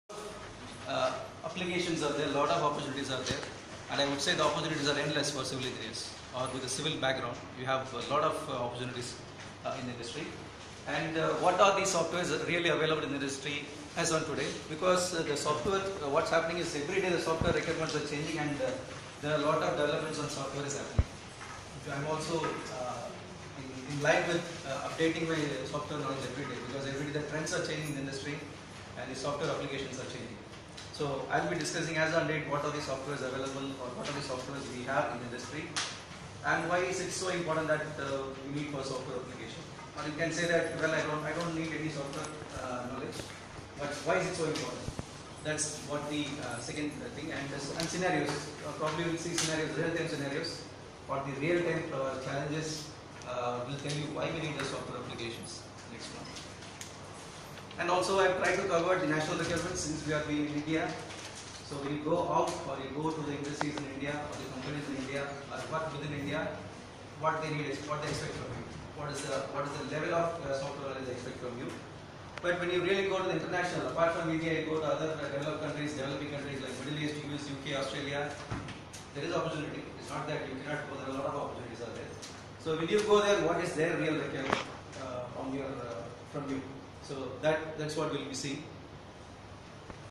ArunBabuAecomBlr 20180915
Er. A. Arun Babu, Senior Engineer, Aecom GDC India Pvt Ltd., Bangaluru delivering a lecture during the workshop on 'Software Application in Civil and infrastructure' on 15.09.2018 at Centre for Water Technology, Department of Civil Engineering, Kalasalingam Academy of Research and Education